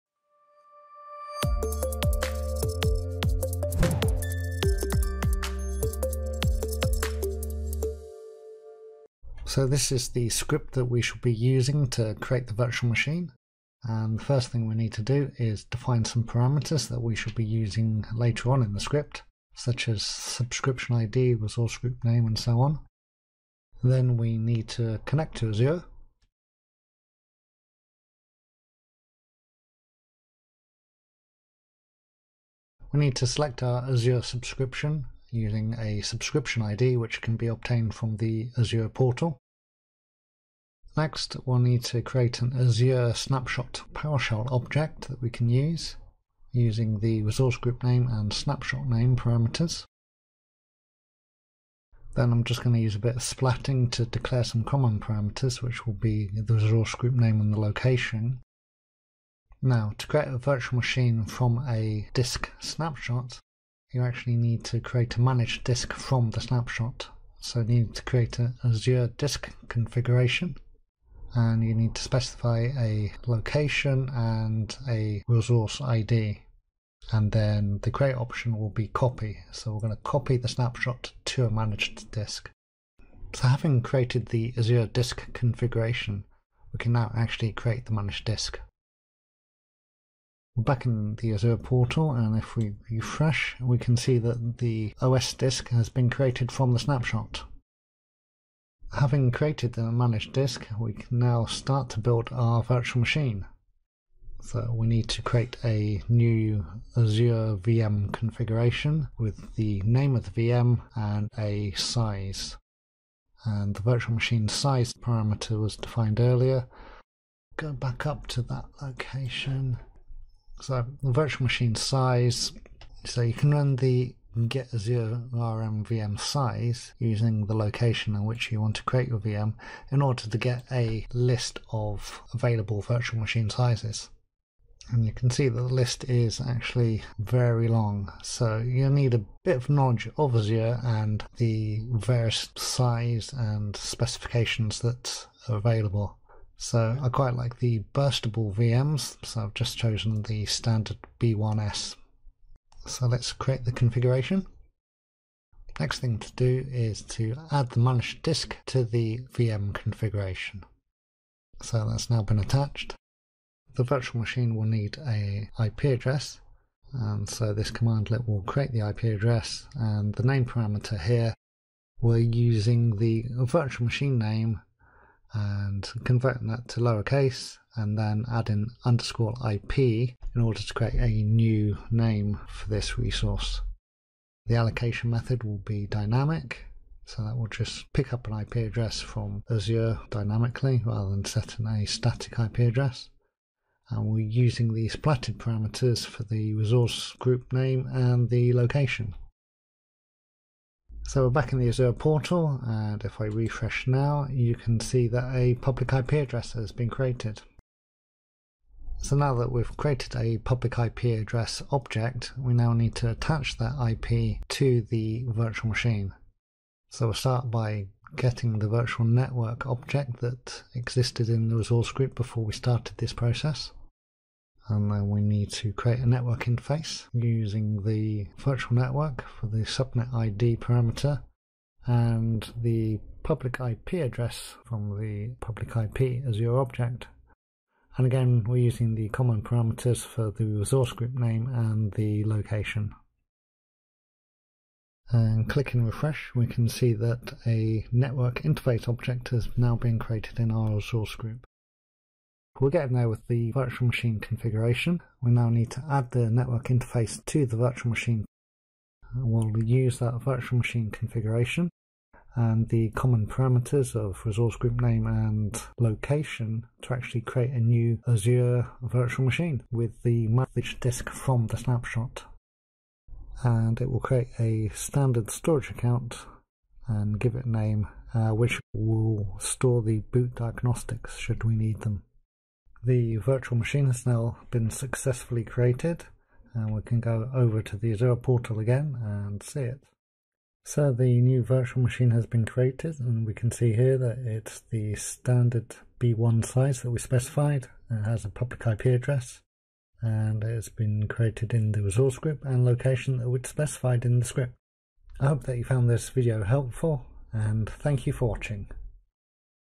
How To Create An Azure Virtual Machine From A Snapshot
In this video Matt will show you how to use a Powershell script to create a new virtual machine in Microsoft Azure from an existing OS disk Snapshot.

An Azure managed disk snapshot is a point-in-time copy of a managed disk. By using the snapshot to create a new virtual machine we can effectively create a copy of an exisiting VM at an earlier point in time without affecting the original or we can programatically create one or many clones of an exisiting VM.

We use the command New-AzureRmDiskConfig to first define a new disk configuration based upon the snapshot and then New-AzureRmDisk to actually create the new managed OS disk.

We then start to build the virtual machine using New-AzureRmVMConfig to define a new VM configuration and Set-AzureRmVMOSDisk to then add the newly created managed OS disk to the VM configuration. Network settings are configured and a NIC added to the VM configuration and then finally we use the command New-AzureRmVM to create thew new virtual machine with an OS disk built from the snapshot.

Pre-requisites
An Azure subscription
An OS disk snapshot
An Azure resource group
An Azure vNet
Azure PowerShell module version 5.1.1

Prerequisites include:
Azure subscription, resource group and vnet.